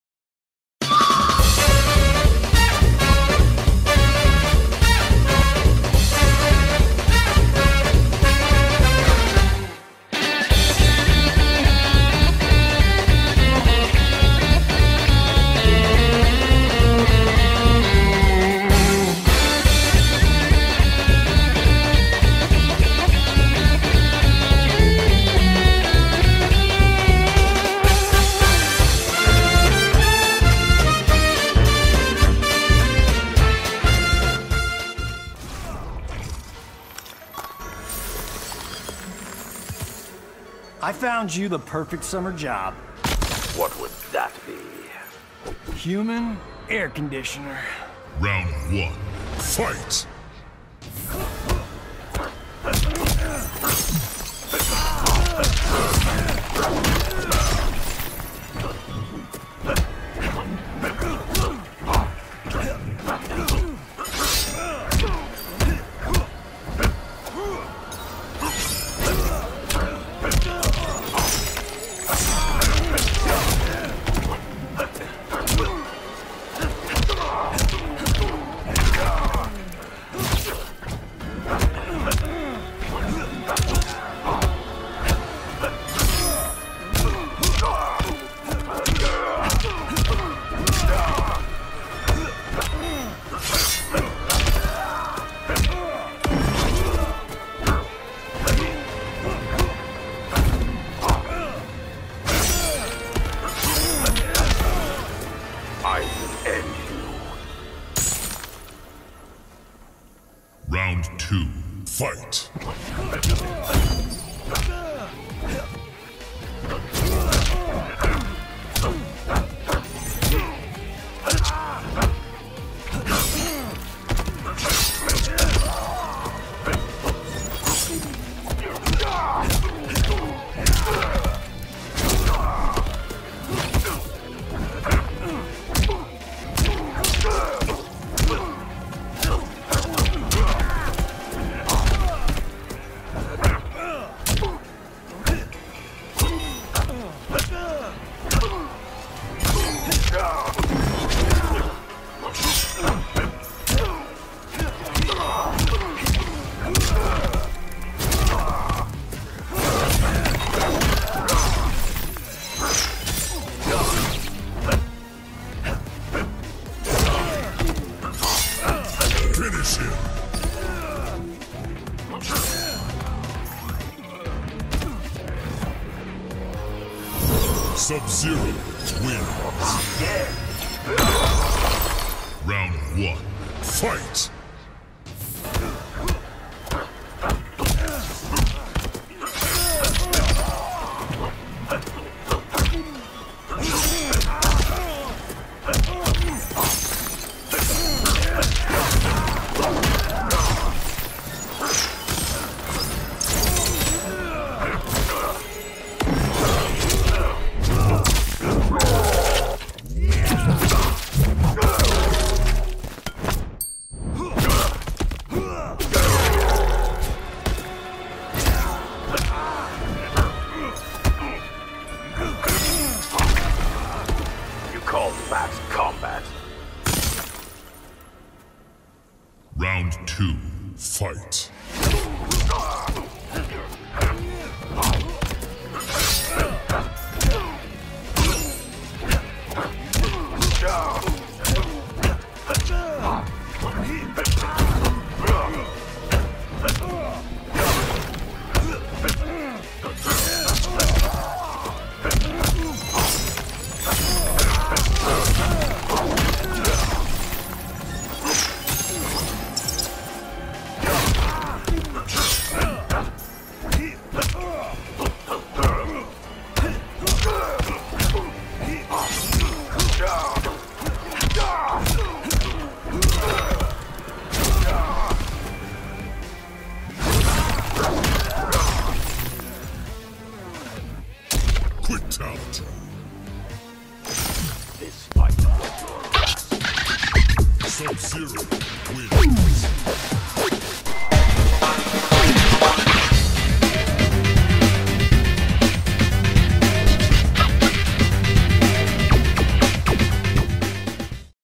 ..shouldn't have said anything
Sub Zero noob destroys trash talking Cage and makes em quit